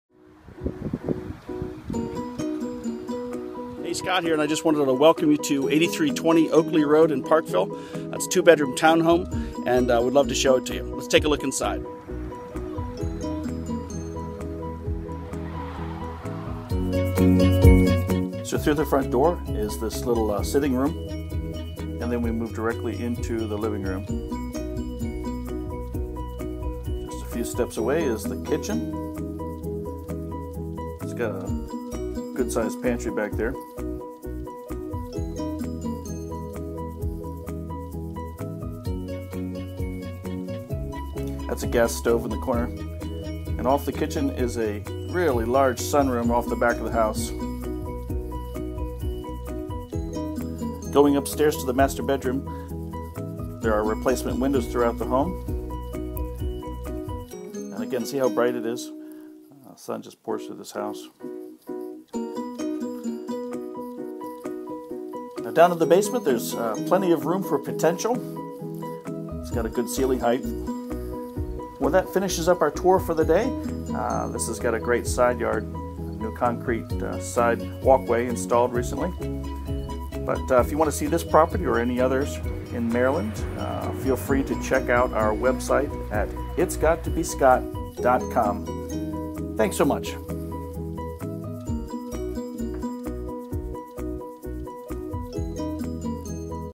8320 Oakleigh Road | Scott Smith Home Selling Team
Are you looking for an investment property?  This is a nice and clean corner Townhome - 8320 Oakleigh Rd in Parkville, MD 21234 with a Sunny Kitchen, large rear Sunroom, Replacement Windows, Central Air Conditioning, a newly poured side concrete walkway and a full basement with lots of potential.